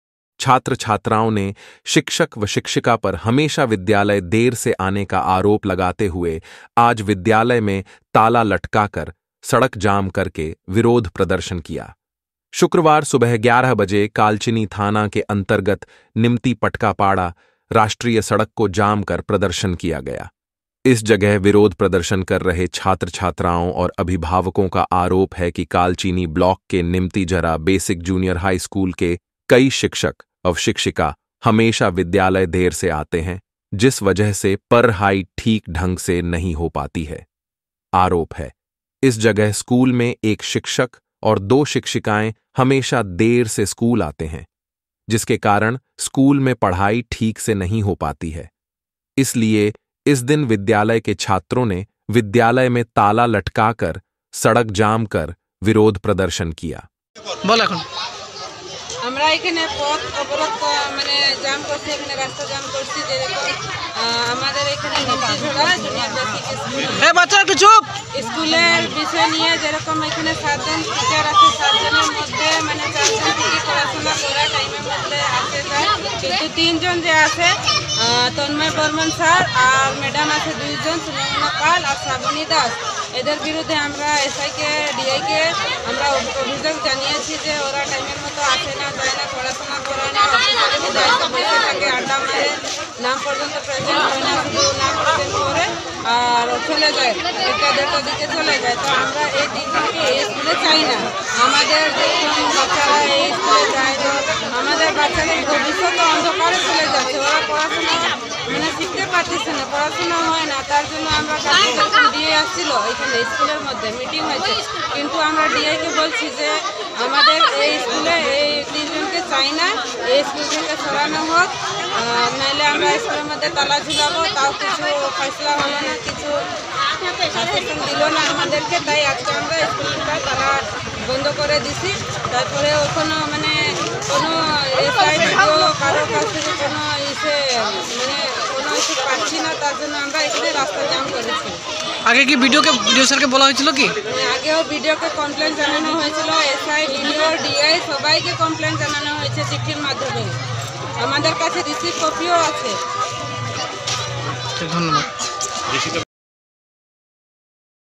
Kalchini विद्यालय में ताला लटका कर सड़क जाम करके विरोध प्रदर्शन किया छात्र-छात्राओं ने
Kalchini शिक्षक व शिक्षिका हमेशा विद्यालय देर से आने का आरोप लगाते हुए विद्यालय में ताला लटका कर सड़क जाम करके विरोध प्रदर्शन किया छात्र-छात्राओं ने